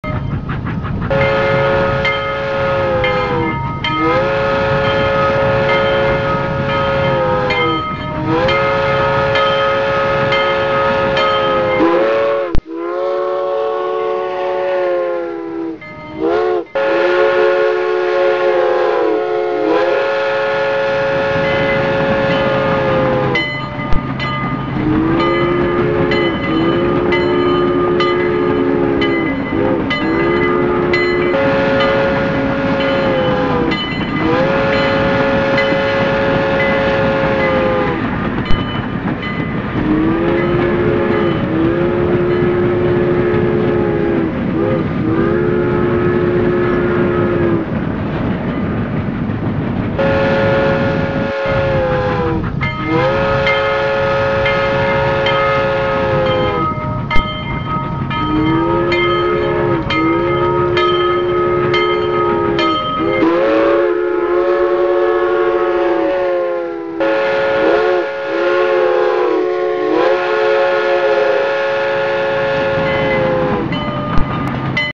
SPETTACOLARE SOLE CHE TRAMONTA DAL TRENO IN CORSA
uno spettacolare sole che tramonta, filmato dal treno verso trieste